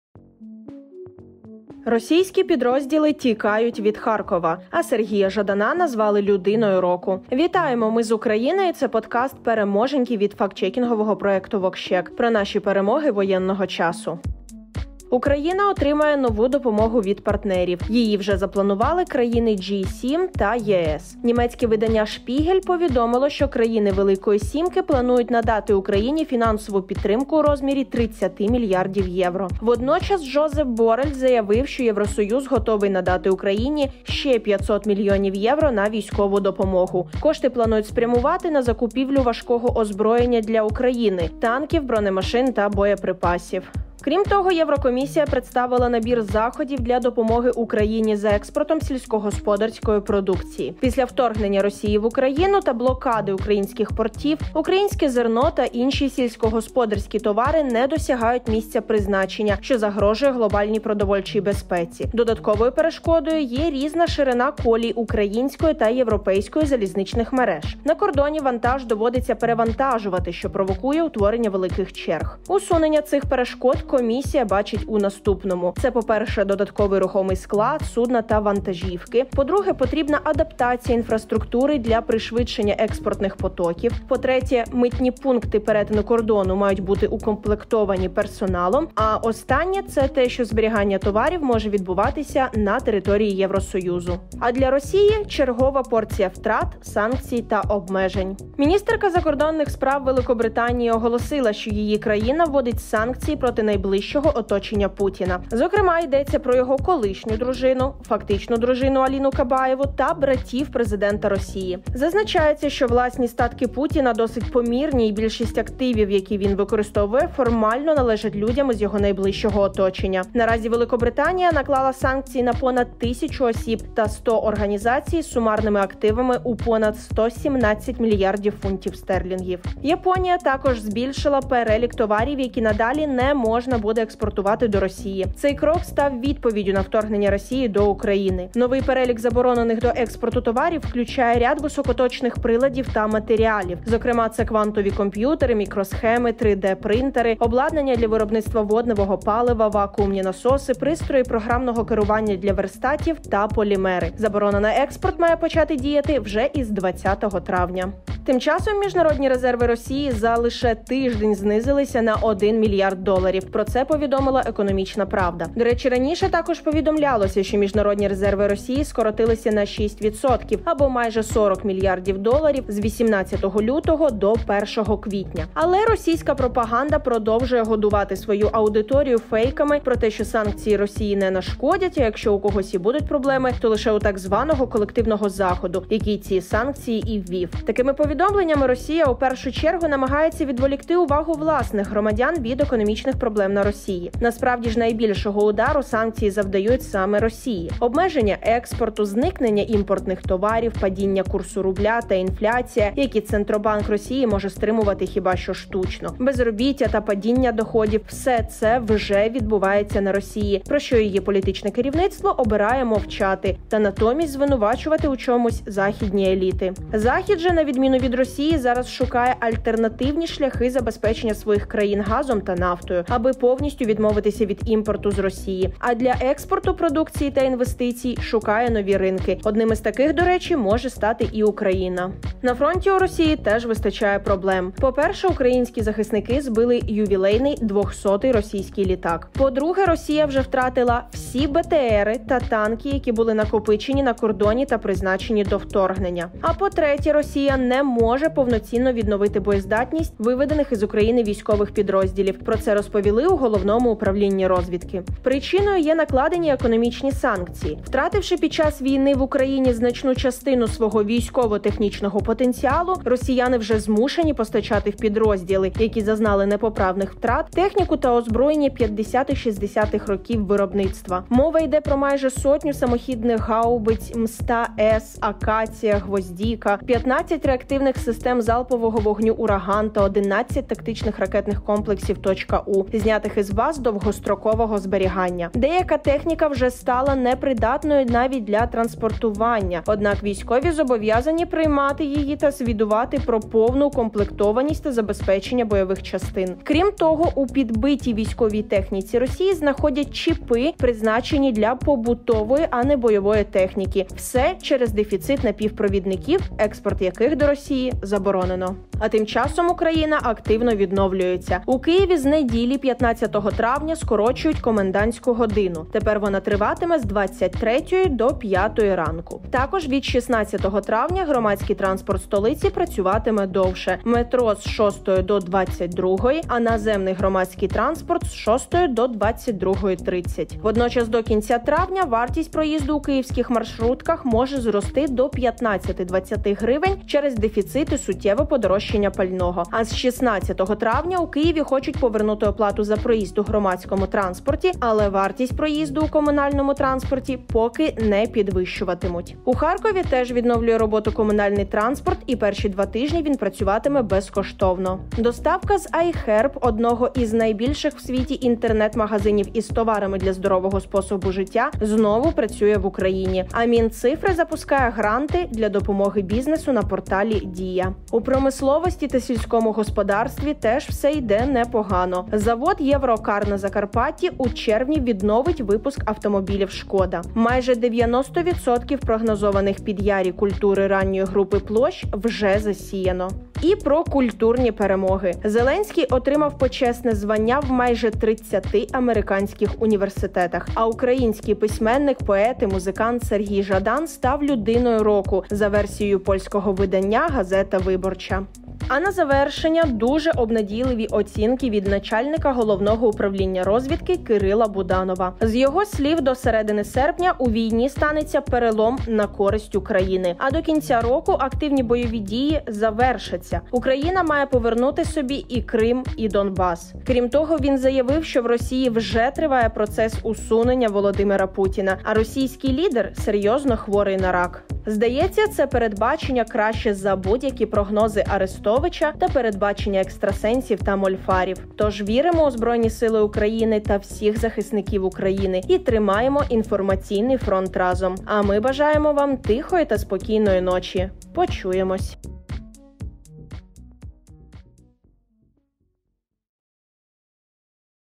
VoxCheck / подкаст Переможеньки 13/05
🔻ЗСУ збили ювілейний 200-ий літак, а ворог вже втратив всі БТРи та танки, які були накопичені на кордоні та призначені до вторгнення.

🔻Міжнародні резерви рф за тиждень знизилися на $1 мільярд. Але російська пропаганда продовжує годувати свою аудиторію “фейками” про те, що санкції росії не нашкодять, а якшо у когось і будуть проблеми — то у так званого колективного Заходу, який ці санкції і ввів.

🔻Обнадійливі оцінки від начальника Головного управління розвідки Кирила Буданова: до середини серпня у війні станеться перелом на користь України, а до кінця року активні бойові дії завершаться. Україна має повернути собі і Крим, і Донбас. У росії вже триває процес усунення володимира путіна, а російський лідер серйозно хворий на рак.